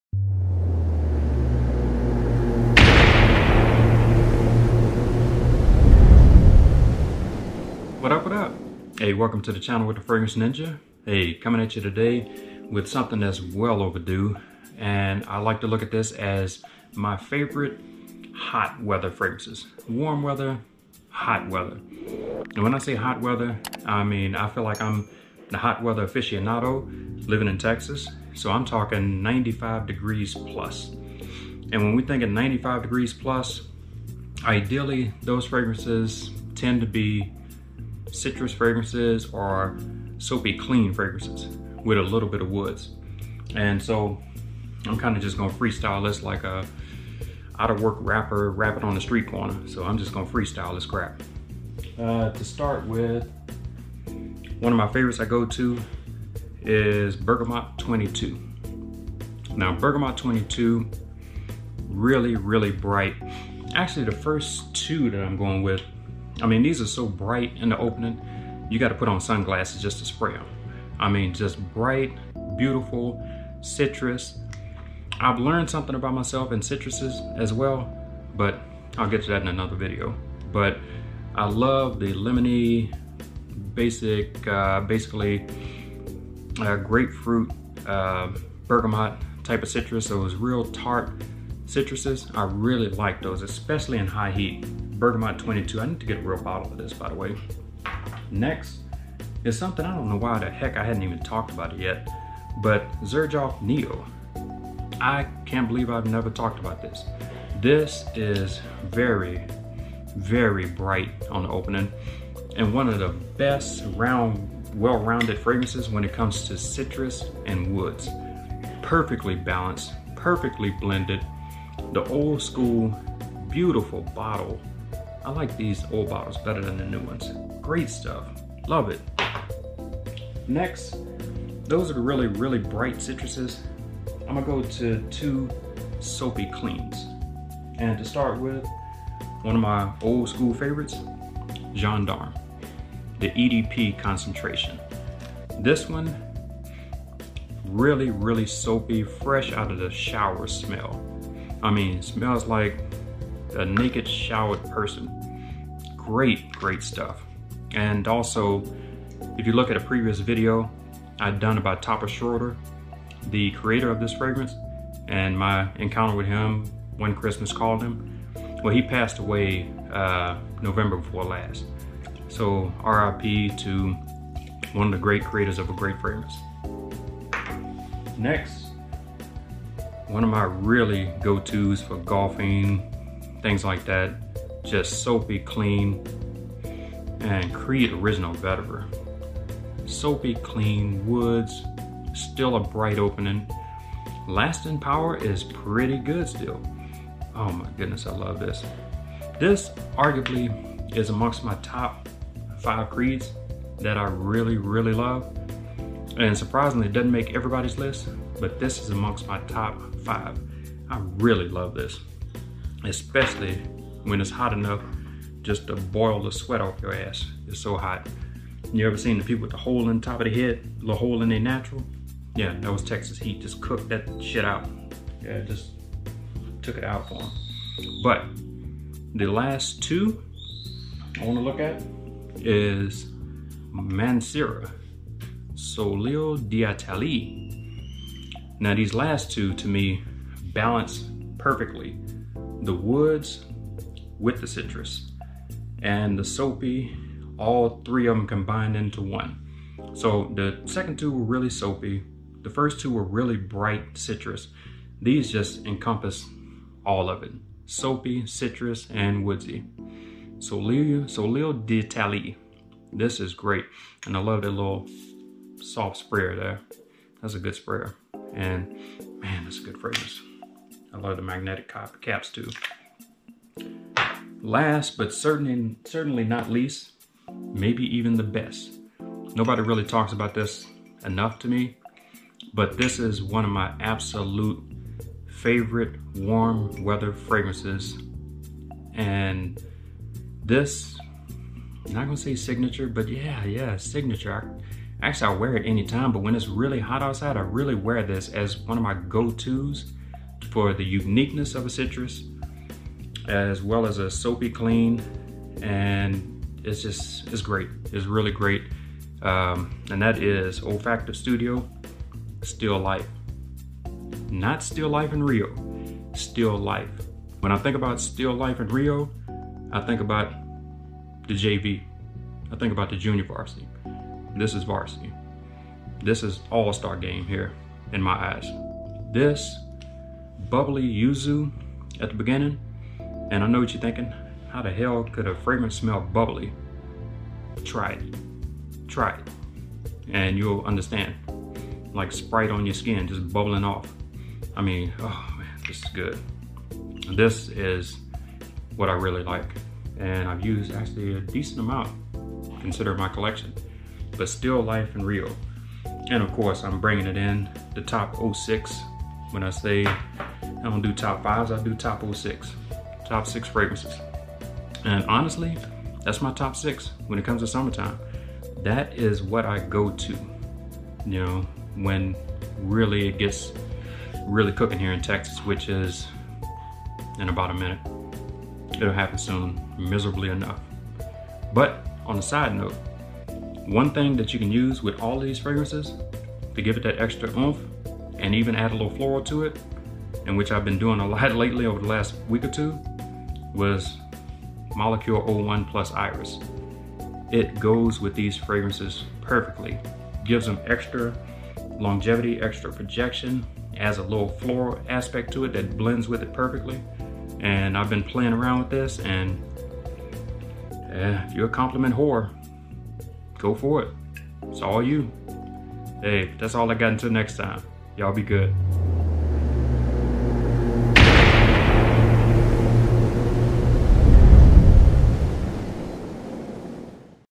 TOP 6 HOT WEATHER FRAGRANCES 🌞 Top 06 in my collection 🌡
My best 6 Hot and Warm weather fragrances in my collection. 06!

Let me know yours!!